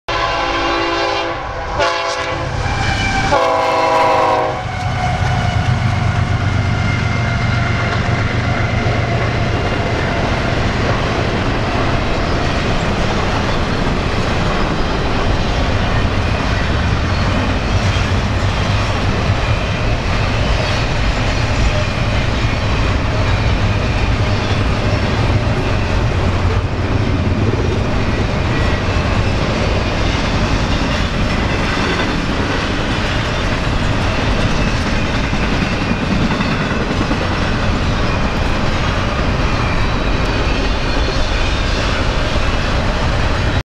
CP 8502 West in Erie, PA 4/15/23
This is the first CPKC train I saw since the merger less than 100 hours prior. Funny how a CN engine was trailing.

CSX I165, Downing Ave., Erie, PA, CSX Erie West Sub
CP 8502 = AC4400CW - CP w/ Beaver - Forward RL K5HL
CN 2774 = ES44AC - Ex CREX